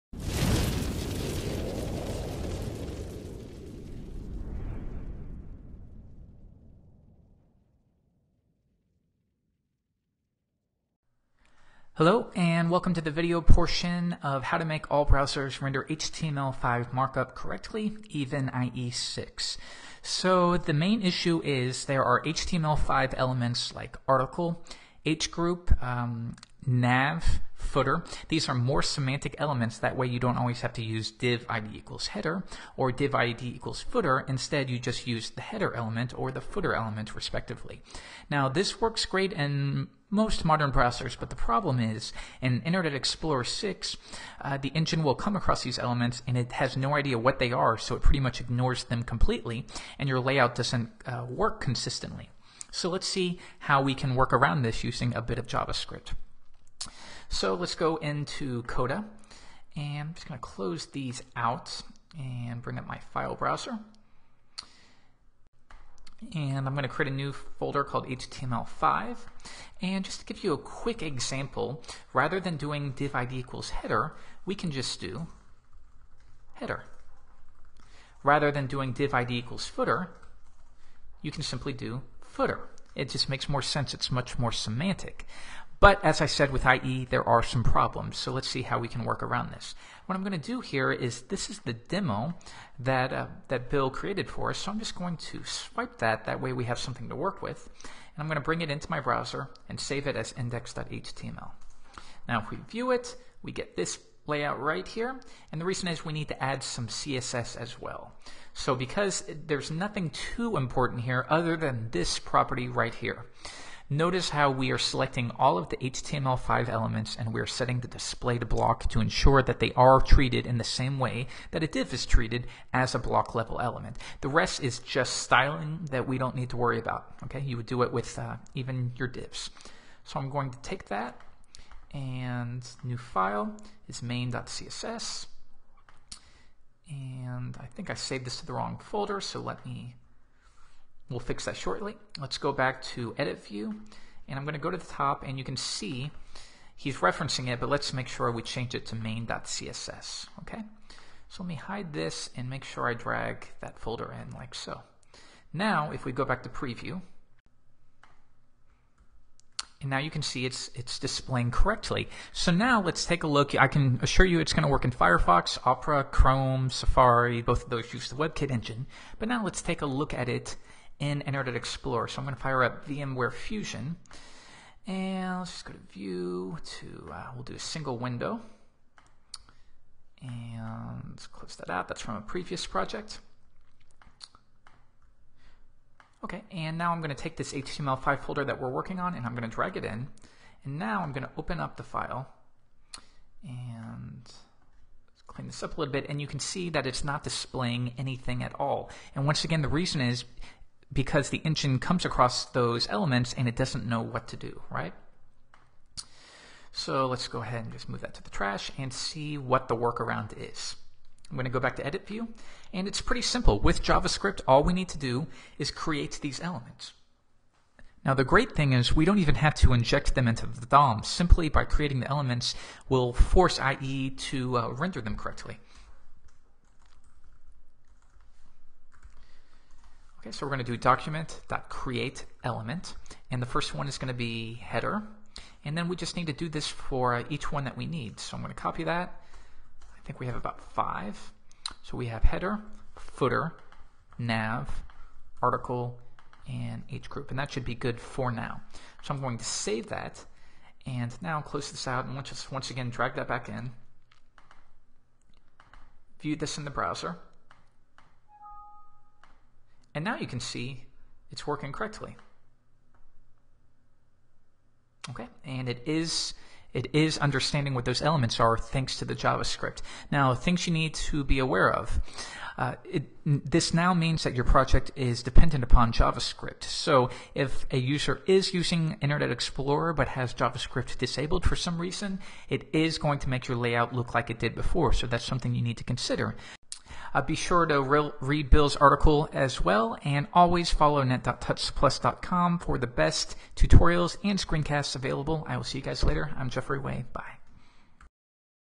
How to Make All Browsers Render HTML5 Mark-up Correctly ? Even IE6
HTML 5 provides some great new features for web designers who want to code readable, semantically-meaningful layouts. However, support for HTML 5 is still evolving, and Internet Explorer is the last to add support. In this tutorial, we?ll create a common layout using some of HTML 5?s new semantic elements, then use JavaScript and CSS to make our design backwards-compatible with Internet Explorer. Yes, even IE 6.